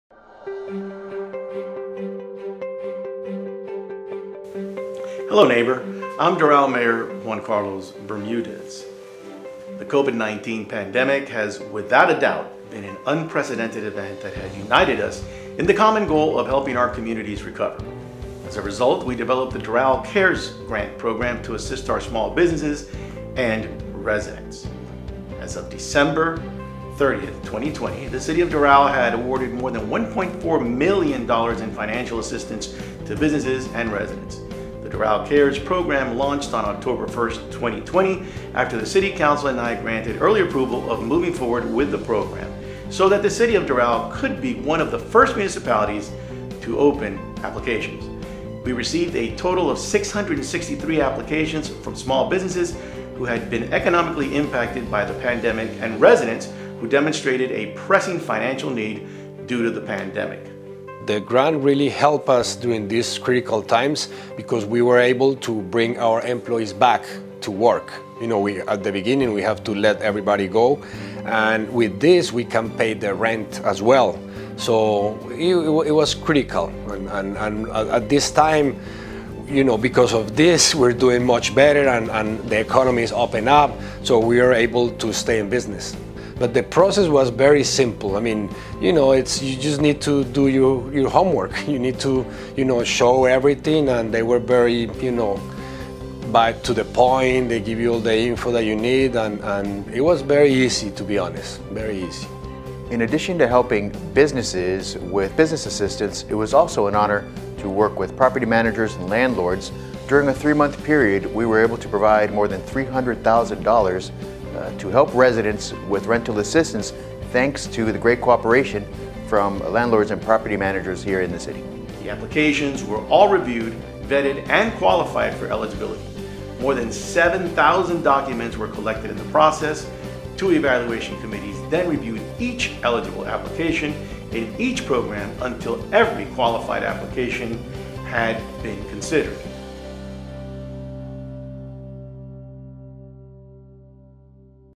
Doral CARES Program Assists Community with Over $1.4 Million in Grants
In response to the COVID-19 pandemic, which economically affected many in our community, Mayor Juan Carlos Bermudez and City Council approved the Doral CARES Program in 2020. Over $1.4 million in grants has since been awarded to eligible and qualified residents and businesses in Doral after collecting several supporting documents and evaluated by committees. Learn more about the process and hear from a business it helped!